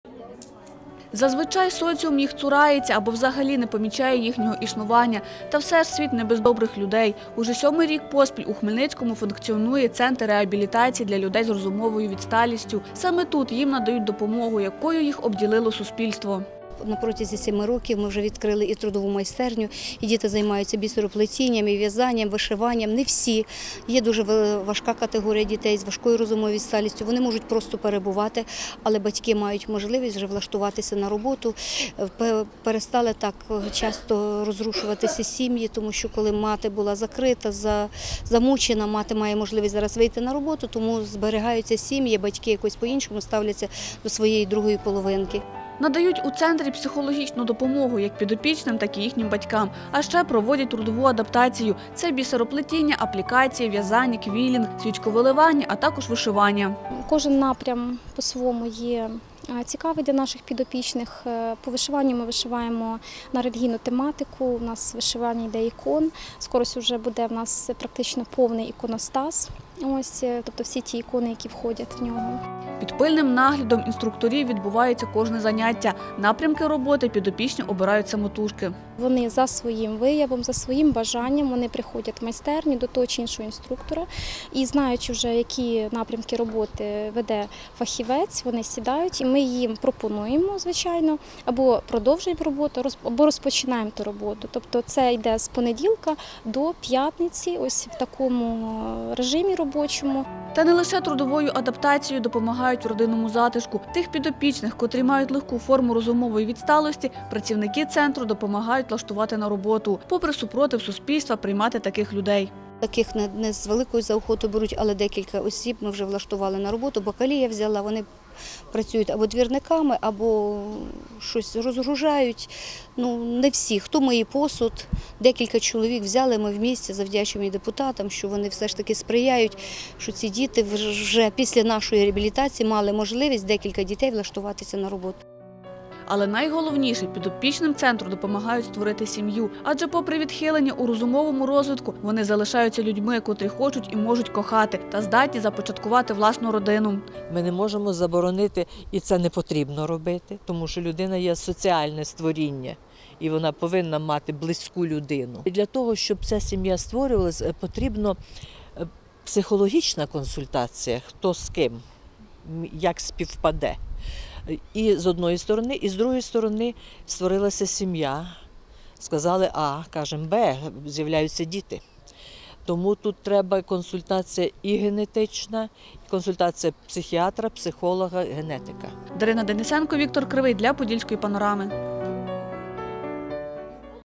ХОДТРК "Поділля-центр" - Родинний затишок
Допомога тим, хто її потребує. У Хмельницькому центрі реабілітації навчають повноцінно жити людей з розумовою відсталістю. Це унікальний заклад, один з перших в Україні, який відвідують молоді інваліди .